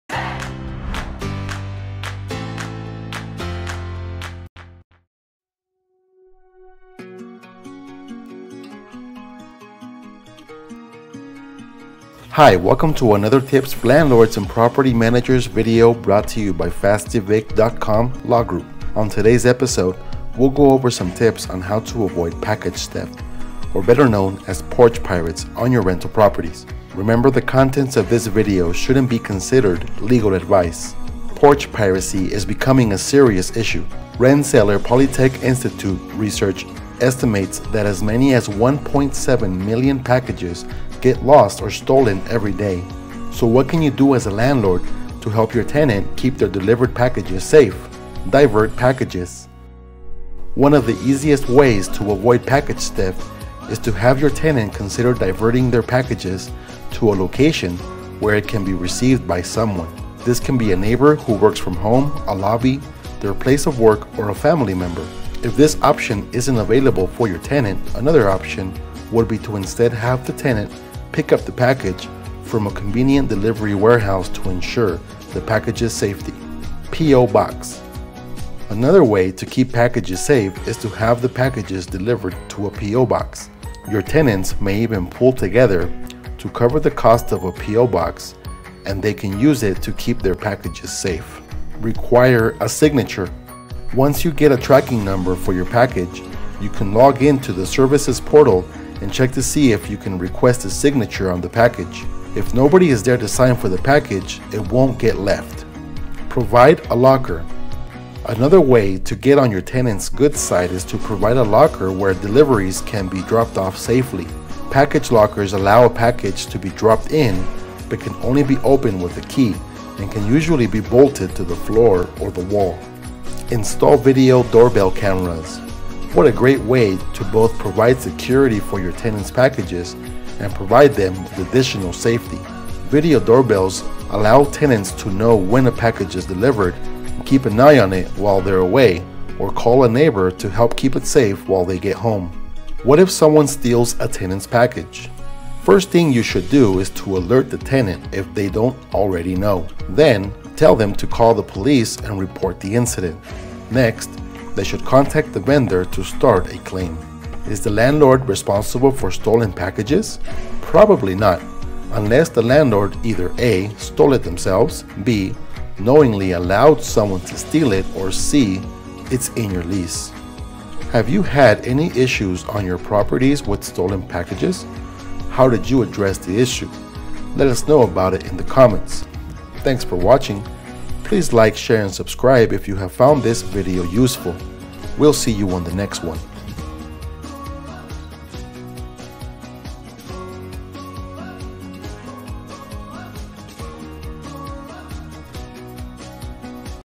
Tips to Avoid Package Theft (Porch Pirates) On Your Rental Properties
Tips on how to avoid package theft (porch pirates)
Hi! On today’s episode, we’ll go over some tips on how to avoid package theft, or better known as porch pirates on your rental properties. Remember, the contents of this video shouldn’t be considered legal advice.

Porch piracy is becoming a serious issue. Rensselaer Polytech Institute research estimates that as many as 1.7 million packages get lost or stolen every day. So what can you do as a landlord to help your tenant keep their delivered packages safe?
Divert packages
One of the easiest ways to avoid package theft is to have your tenant consider diverting their packages to a location where it can be received by someone. This can be a neighbor who works from home, a lobby, their place of work or a family member.

If this option isn’t available for your tenant, another option would be to instead have the tenant pickup the package from a convenient delivery warehouse to ensure the package’s safety.
Require a Signature
Once you get a tracking number for your package, you can log into the portal and check to see if you can request a signature on the package. If nobody is there to sign for the package, it won’t get delivered!
Provide a locker
Another way to get on your tenant’s good side is to provide a locker where deliveries can get dropped off safely. Package lockers allow a package to be dropped in but can only be opened with a key and can usually be bolted to the floor or a wall.
Install video doorbell cameras
What a great way to both provide security for your tenant’s packages and provide them with additional safety. Video doorbells allow tenants to know when a package is delivered and keep an eye on it while away or call a neighbor to help keep it safe while they get home.
What if someone steals a tenant’s package?
First thing you should do is to alert the tenant (if they don’t know already). Then, tell them to call the police to report the incident. Next, they should contact the vendor to start a claim.
Is the landlord responsible for stolen packages?
Probably not, unless the landlord either a.) stole it themselves b.) knowingly allowed someone to steal it or c.) it’s in your lease.

Have you had any issues on your properties with stolen packages? How did you address the issue? Let us know about it in the comments!